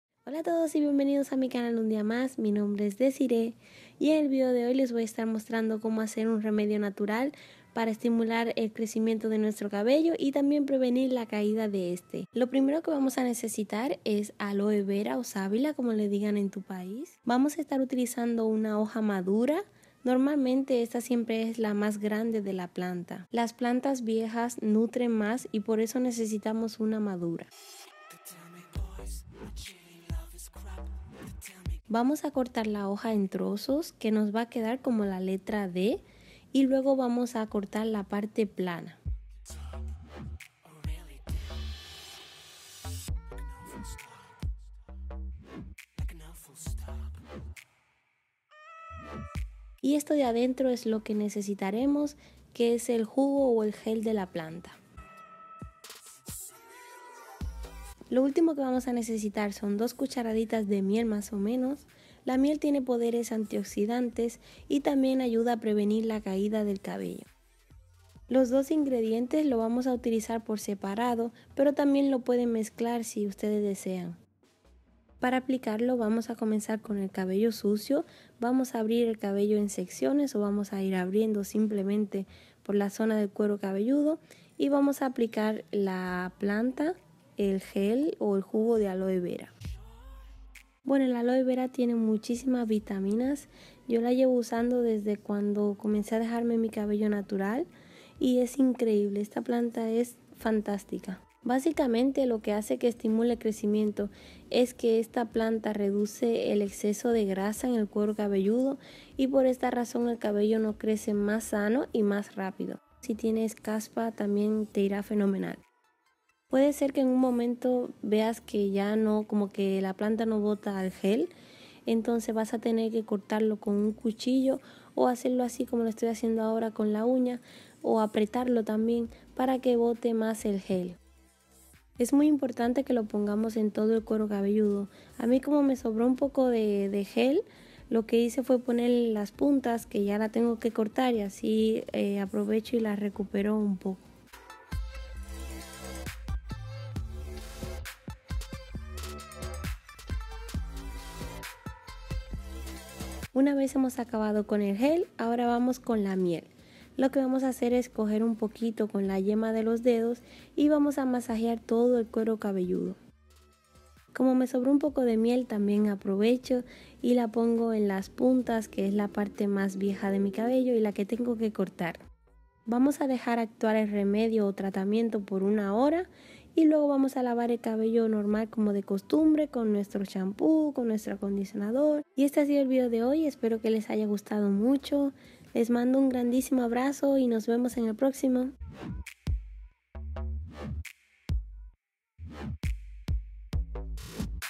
Remedio natural para hacer crecer el cabello | FRIZZYDESI
Hola,

En el video de hoy les muestro como hacer un remedio natural para hacer crecer el cabello rápido y sano. Los que siguen el reto podemos hacerlo hasta tres veces en esta semana.